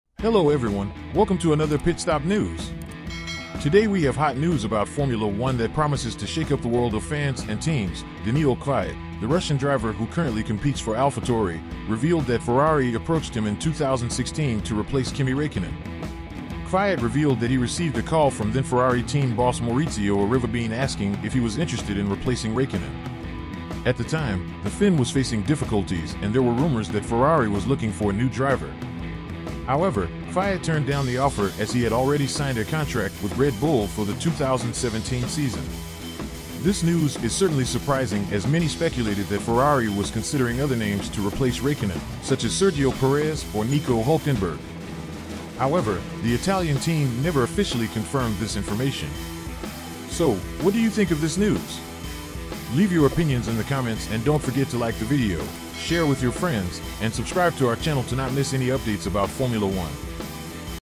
Ferrari considered Kvyat as possible replacement for Raikkonen in 2016
Hello everyone! Today we have hot news about Formula 1: Daniil Kvyat, the Russian driver, revealed that Ferrari approached him in 2016 to replace Kimi Raikkonen. Kvyat revealed that he received a call from then-Ferrari team boss Maurizio Arrivabene asking if he was interested in replacing Raikkonen. However, Kvyat turned down the offer as he had already signed a contract with Red Bull for the 2017 season.